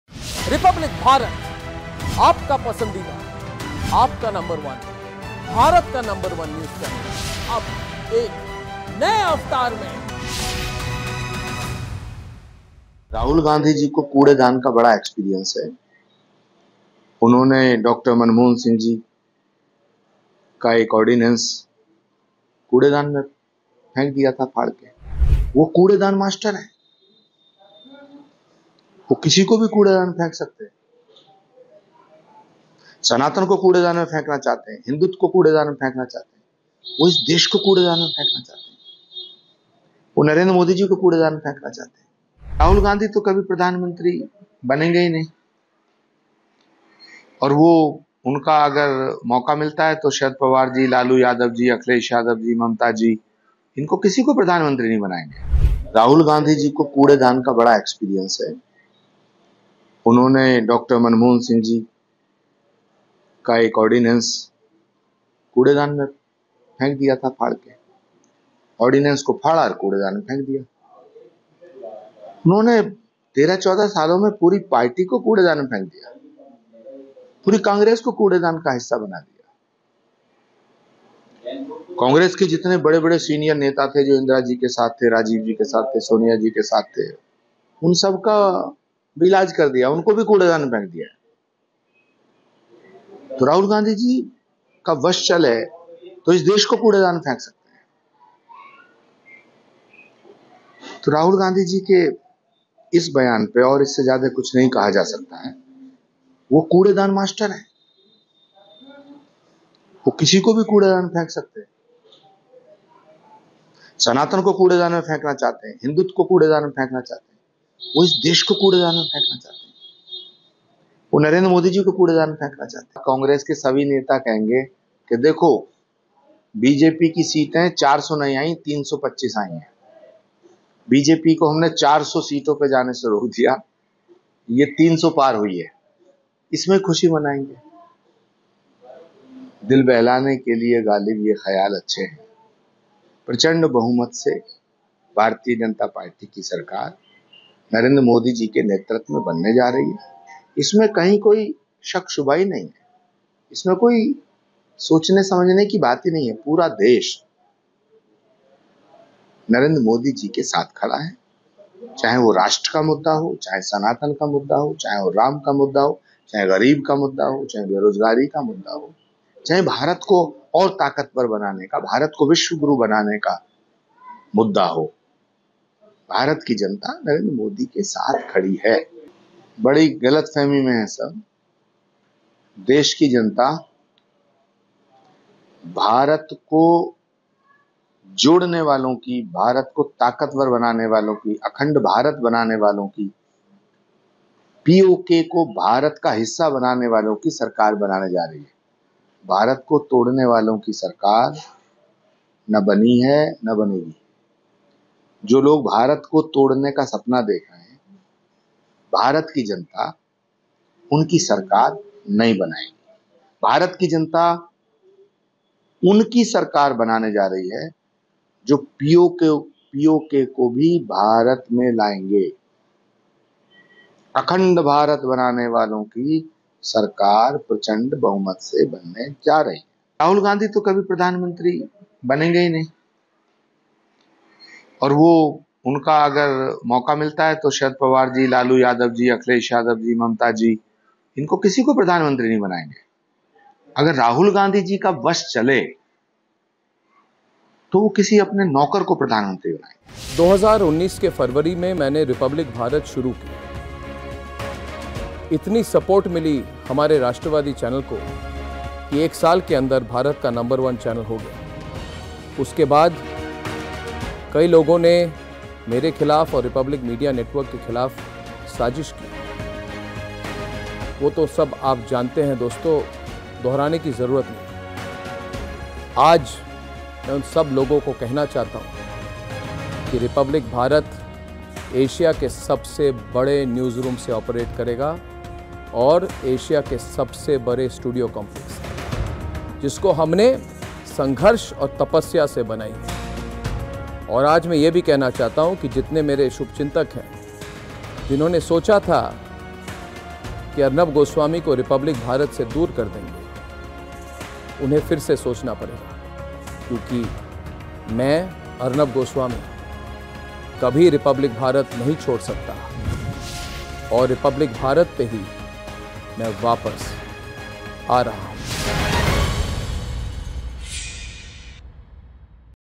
Rahul Gandhi पर Acharya Pramod Krishnam ने कसा तंज, कांग्रेस नेता को बताया "कूड़ेदान मास्टर"
राहुल गांधी के बयान पर पूर्व कांग्रेस नेता आचार्य प्रमोद कृष्णम ने कहा कि राहुल गांधी को कूड़ेदान का बड़ा अनुभव है। उन्होंने मनमोहन जी का एक ऑर्डिनेंस फाड़ कर कूड़ेदान में फेंक दिया था। उन्होंने 13-14 साल में पूरी कांग्रेस को कूड़ेदान में फेंक दिया। कांग्रेस के बड़े-बड़े नेता जो सोनिया जी, राजीव जी, इंदिरा जी के साथ थे उन सब को भी कूड़ेदान में फेंक दिया। राहुल गांधी की चले तो इस देश को कूड़ेदान में फेंक दें। वे कूड़ेदान मास्टर हैं। वे किसी को भी कूड़ेदान में फेंक सकते हैं। सनातन को कूड़ेदान में फेंकना चाहते हैं।